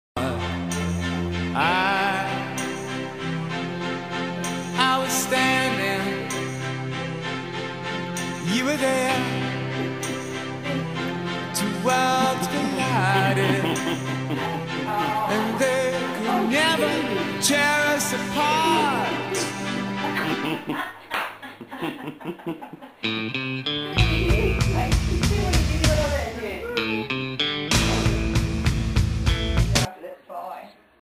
Cat and dog kissing funny
Matrix the husky is kissing the cat